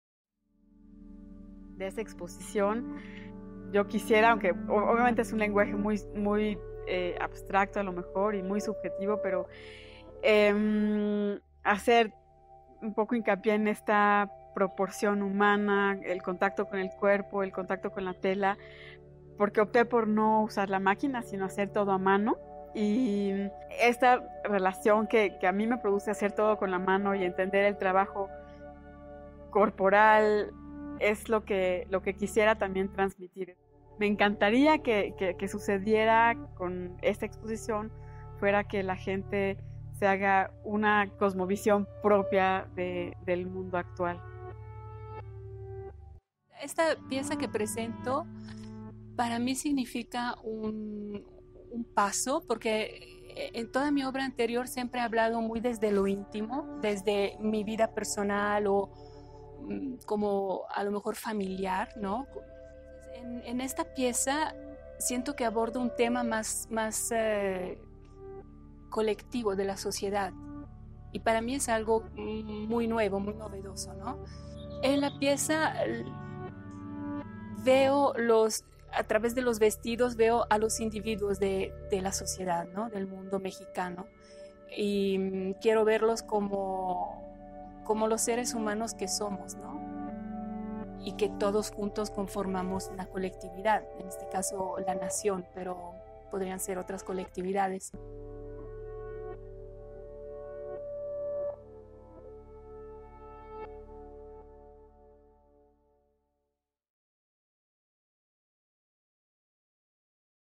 De bordados y tejedoras: Patrones en el tiempo
Ornella Ridone y Sandra Contreras son artistas visuales fieles a su oficio de bordadoras y tejedoras. La primera, italiana de origen y radicada en México desde los noventa, vive y trabaja en el estado de Oaxaca. La segunda, mexicana, vive y trabaja desde el 2001, en Berlín, Alemania.

Ambas reúnen su trabajo en Patrones de tiempo y espacio que se inauguró el miércoles 28 de febrero a las 19:30 horas en la Celda Contemporánea de la Universidad del Claustro de Sor Juana.